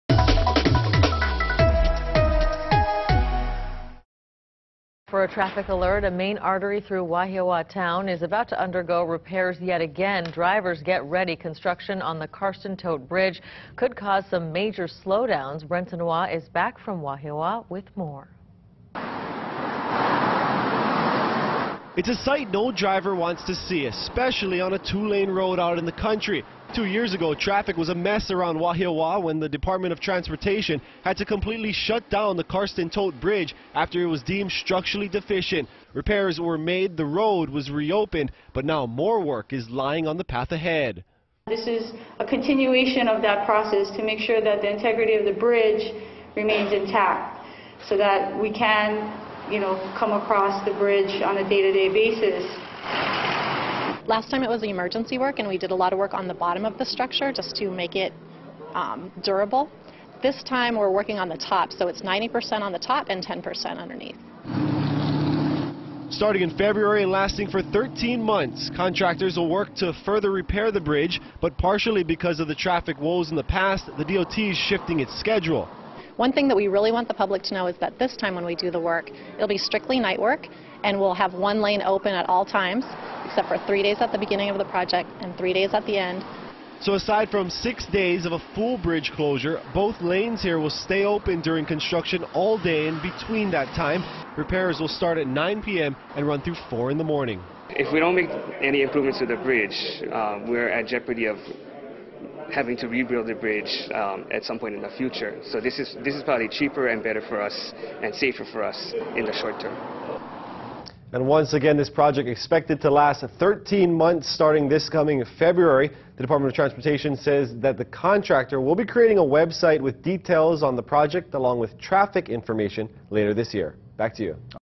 Karsten Thot bridge to undergo repairs in 2015
The main artery for Wahiawa, the Karsten Thot bridge will be having repairs beginning early 2015.